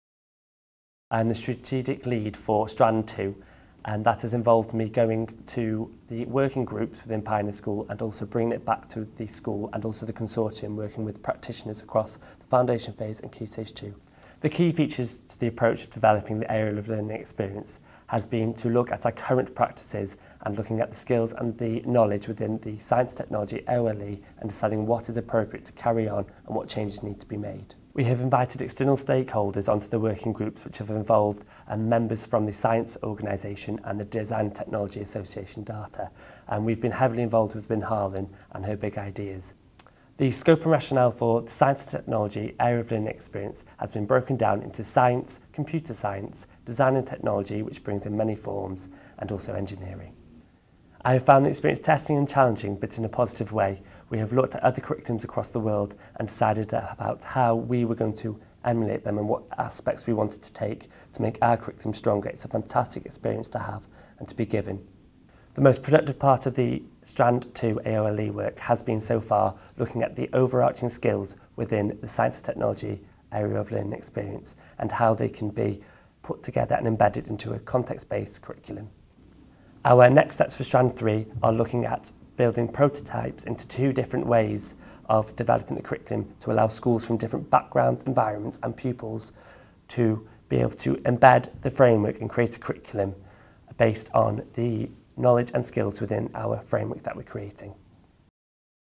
Adam Shepherd – Science and Technology Area of Learning and Experience
Developing a new Curriculum for Wales – teacher Adam Shepherd, Ysgol T Gwynn Jones, talks about progress with Science and Technology